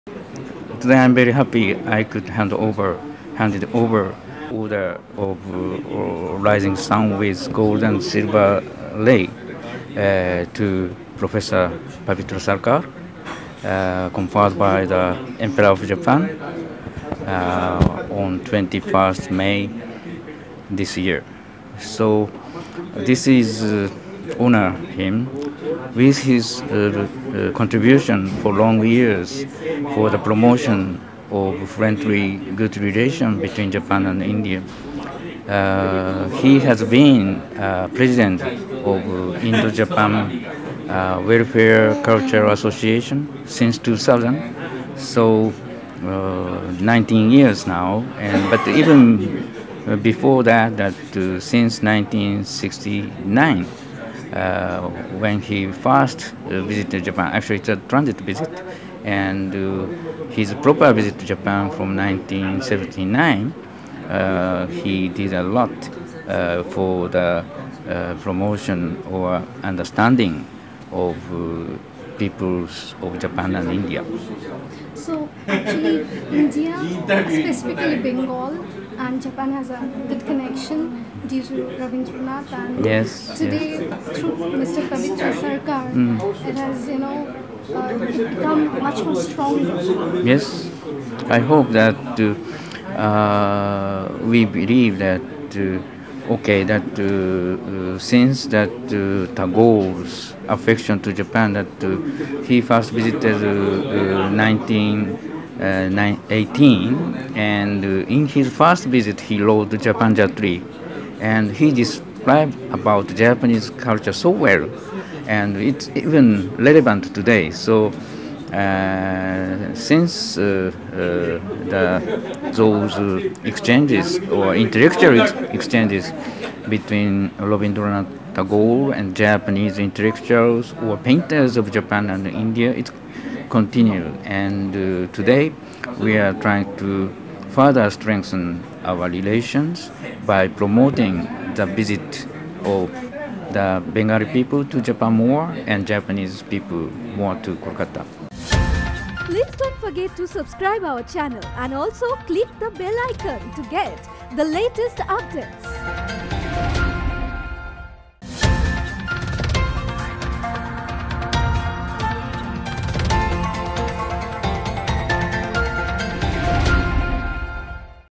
PABITRA SARKAR CONFERRED WITH 'THE RISING SUN GOLD AND SILVER RAY' BY JAPAN | ANM NEWS | 160719
Indo-Japan bilateral relation reached a new high when noted educationist Pabitra Sarkar was conferred the order of the rising run gold and silver ray. The order akin to Padmasree in India, was decorated on Dr Sarkar by Consul general of Japan Masayuki Taga at a simple ceremony in his official residence in south Kolkata. ANM News spoke exclusively to Mr Taga.